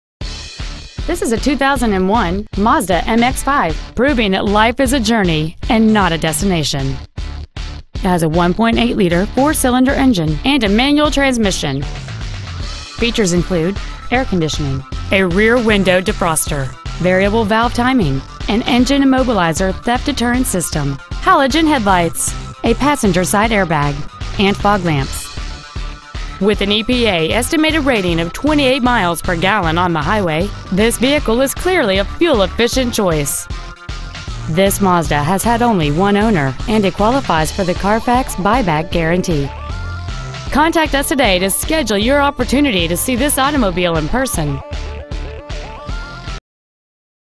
Pre-Owned 2001 Mazda MX-5 Miami FL
We are proud to present this 2001 Mazda MX-5 .

 Year : 2001
 Make : Mazda
 Model : MX-5
 Engine : gas i4 1.8l/110
 Trans . : manual
 Exterior : black
 Miles : 87,211
 Interior : tan

 Lehman Mazda